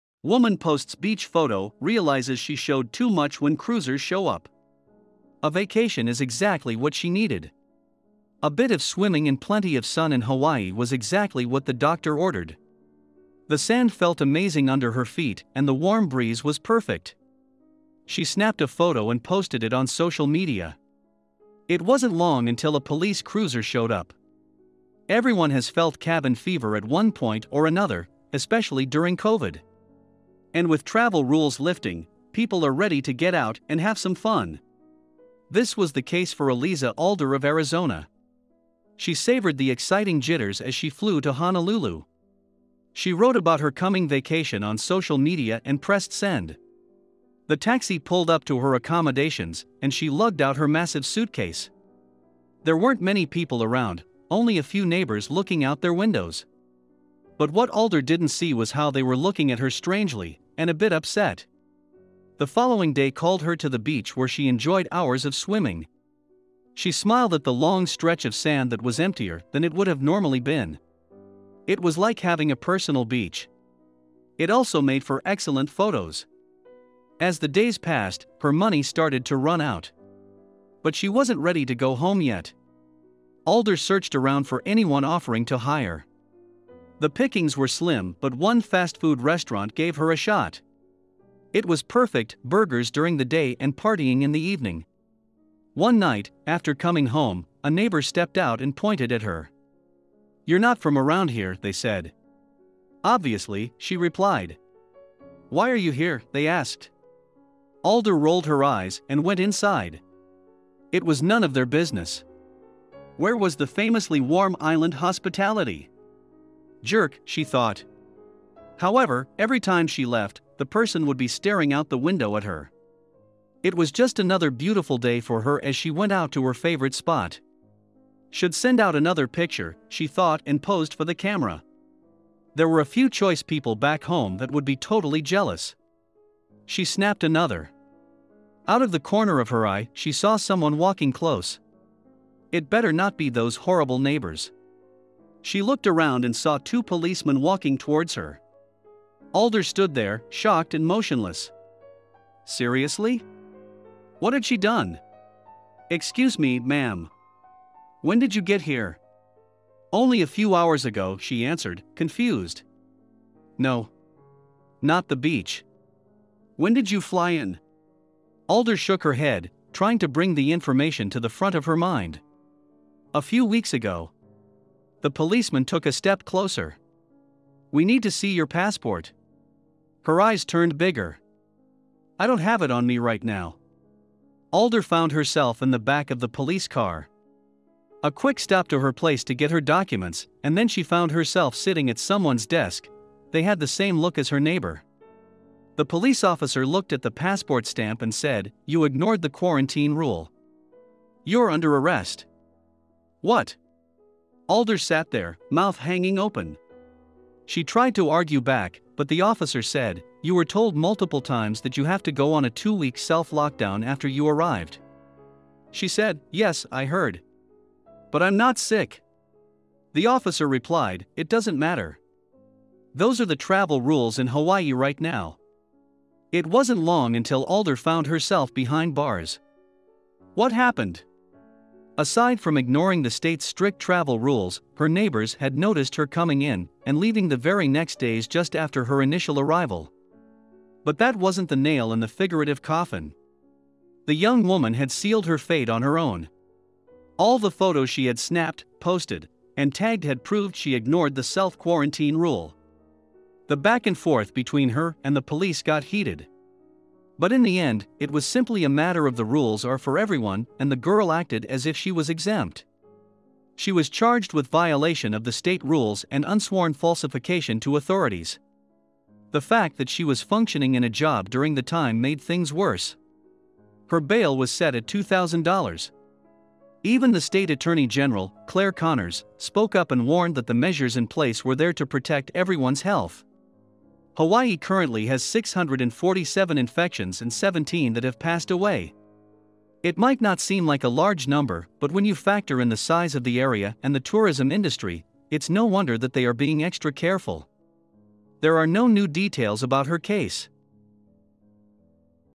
Woman Posts Beach Photo, Realizes She Showed Too Much When Cruisers Show Up
She felt like what she needed most was a break from the stress of everyday life. It would help if that included swimming in the ocean and sunbathing on the beaches of Hawaii.

She could feel every particle of sand between her toes as she felt the gust of warm air rolling over the beach. She decided to take a photo on social media. Soon after posting it, she was met by a police cruiser.

Everyone can feel cooped up in their houses from time to time and when a pandemic like COVID forces you to stay inside for months, we can sympathize.

Eventually, with travel rules slackened, people are rushing to get out of their homes and enjoy some much needed time off. Alyza Alder from Arizona was one of those people. She couldn’t contain her excitement as she landed in Honolulu. She was documenting her vacation through social media.